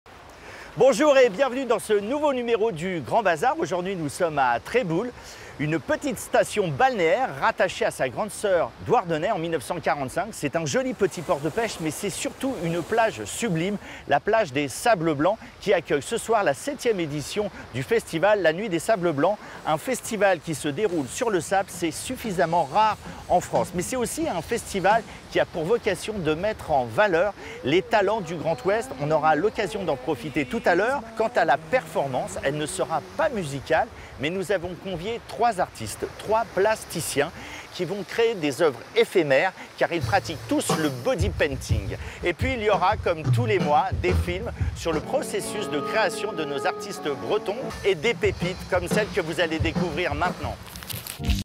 Alexandre Pesle pour le Grand Bazh.Art à Dournenez
Emission spéciale pour la 7ème édition de la Nuit des Sables Blancs